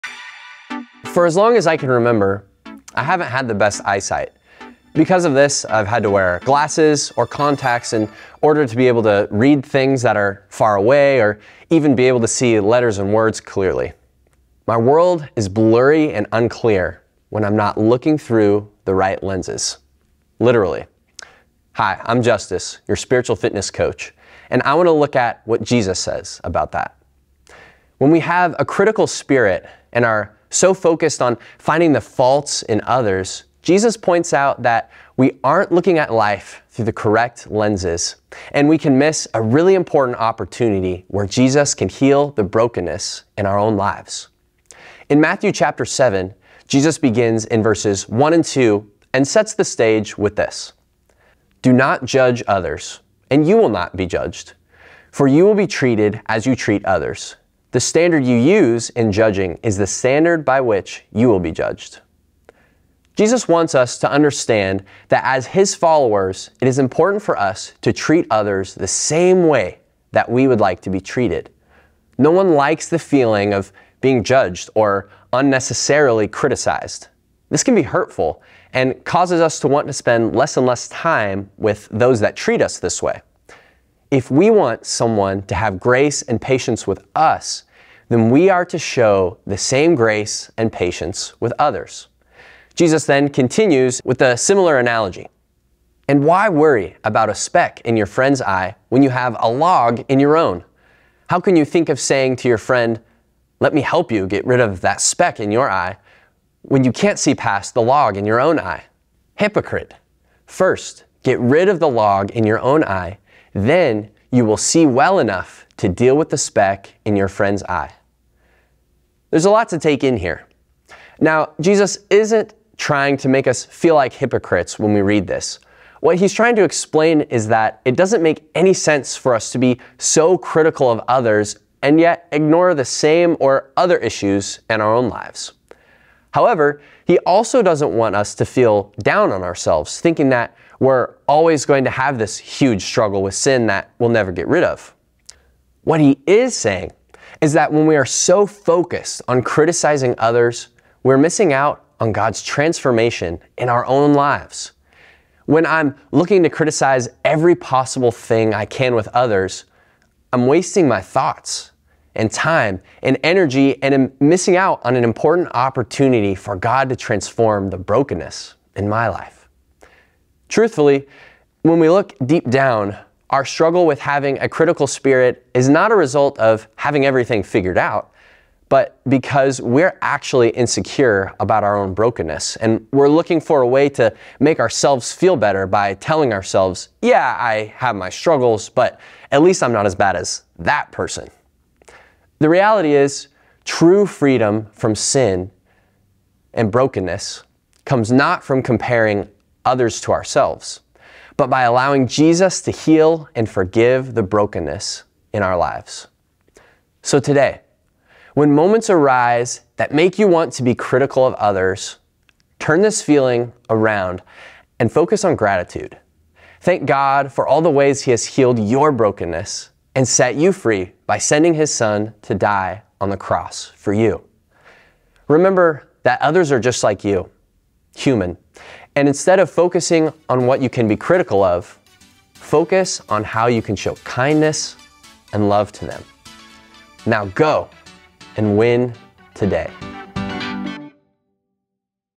Dial in the Focus - Win Today
How well are you seeing others? Yourself? Coach Justus helps us dial in the focus so we won’t miss the opportunities Jesus offers to heal the brokenness in our own lives as well as in the lives of others.